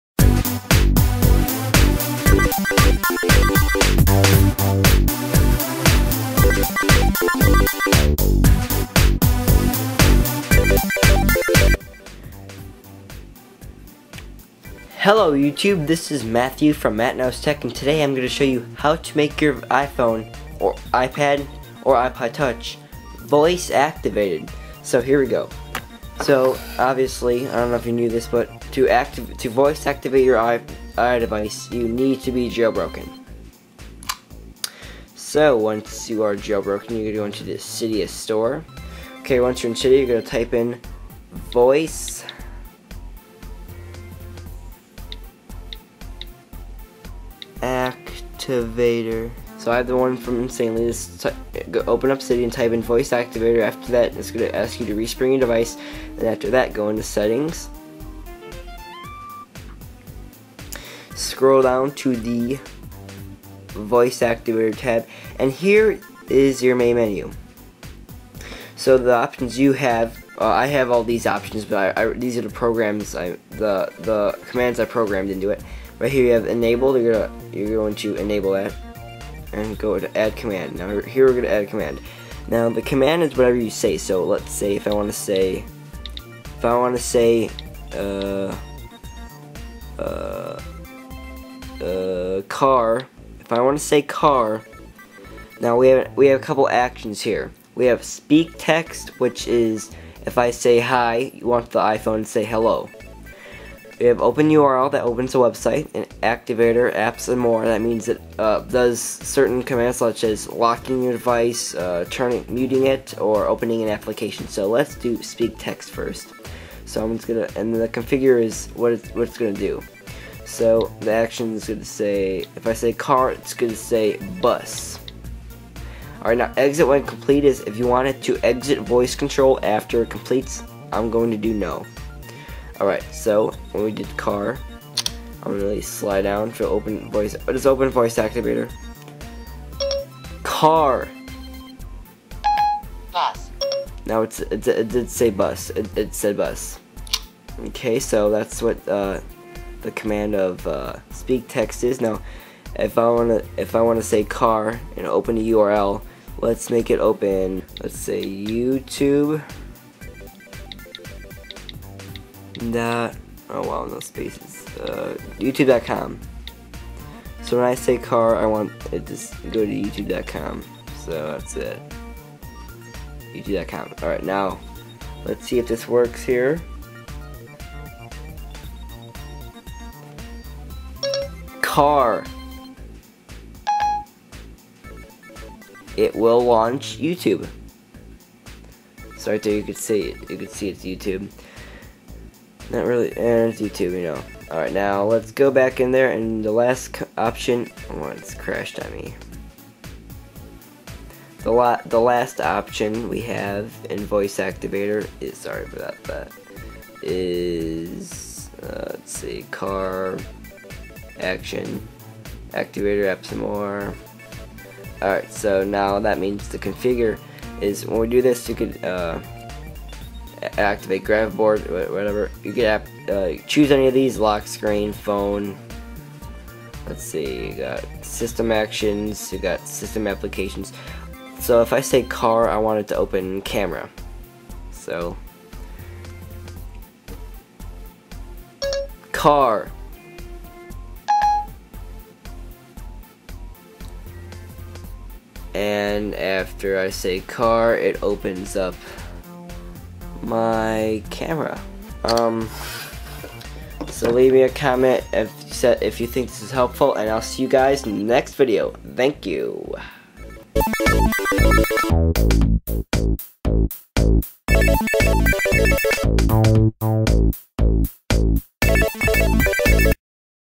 How to Voice Activate your iPhone and iPod Touch!
VoiceActivator Tweak from Cydia.  Top and Best Tweak from Cydia of 2011!!